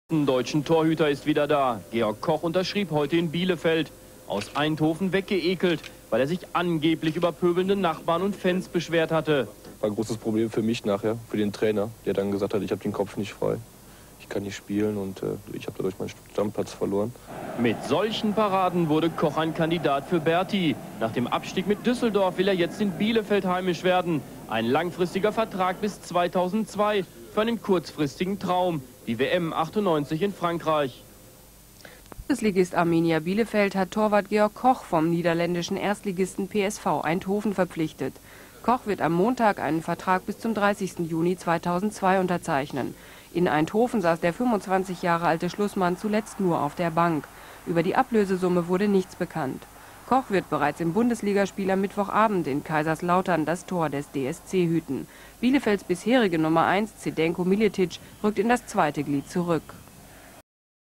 Georg Koch in Bielefeld als Neuzugang vorgestellt (10/97)
Torwart Geord Koch, ehemals Fortuna Düsseldorf, wechselt vom PSV Eindhoven zu Arminia Bielefeld, löst dort Zdenko Miletic als Nr. 1 ab.